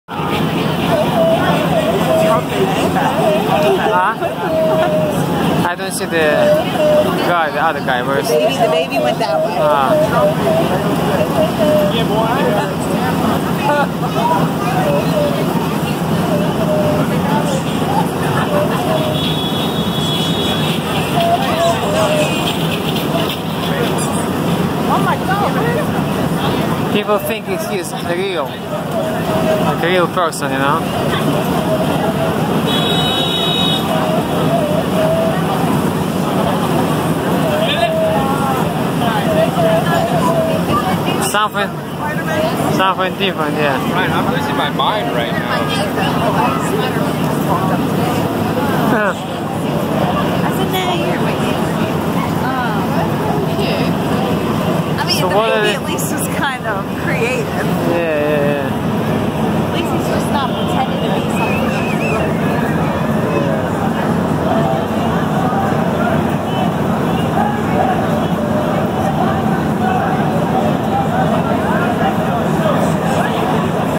Superheroes in Times Square
Times Square is well known for its various Superheroes and Disney characters. You can meet and take a photo with them.